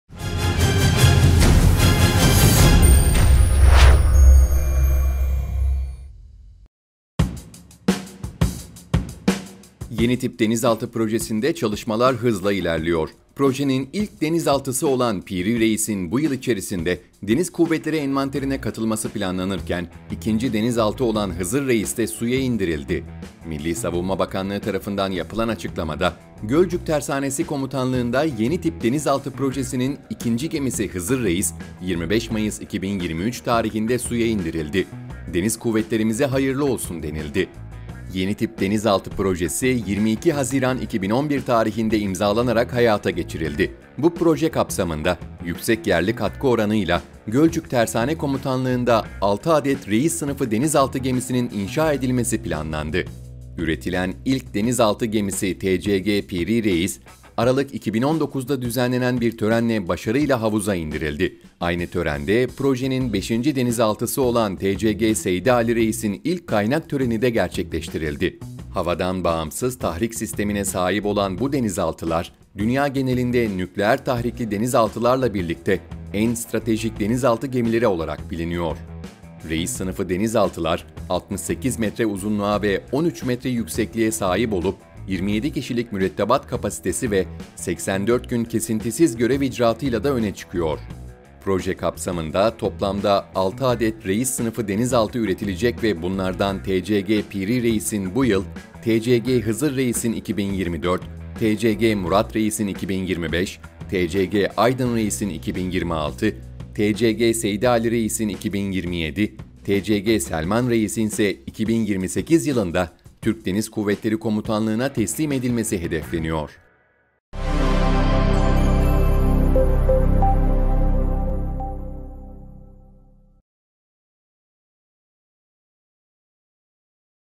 Dünyanın en stratejik denizaltıları olacaklar
denizaltı millisavunma hulusiakar savunmasanayii Yeni Tip Denizaltı Projesi'nin ilk denizaltısı olan TCG PİRİ REİS'in bu yıl içerisinde deniz kuvvetleri envanterine katılması planlanırken, ikinci denizaltı olan TCG HIZIR REİS de suya indirildi.